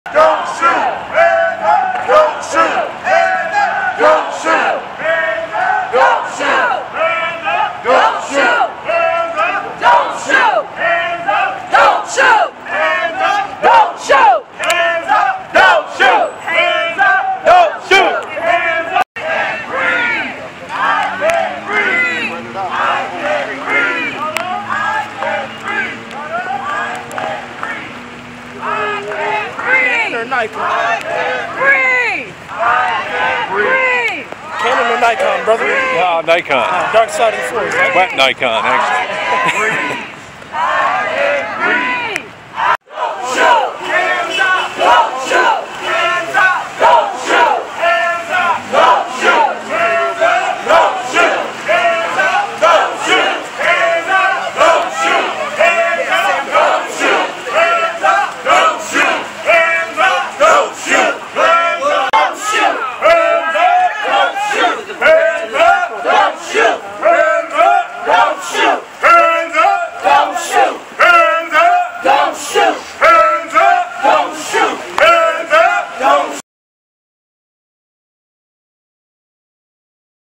NAACP Journey to Justice Rally New Britain Connecticut
12/6/2014 Mike Orazzi | Staff
Ronald Davis, president of the New Britain NAACP chapter during the “Journey to Justice Rally” Saturday afternoon in downtown while spotlighting the recent grand jury decision not to indict a white police officer in the death of Ferguson, Mo. teenager Michael Brown and the videotaped chokehold death of Eric Garner, an unarmed black man stopped for selling loose, untaxed cigarettes in New York.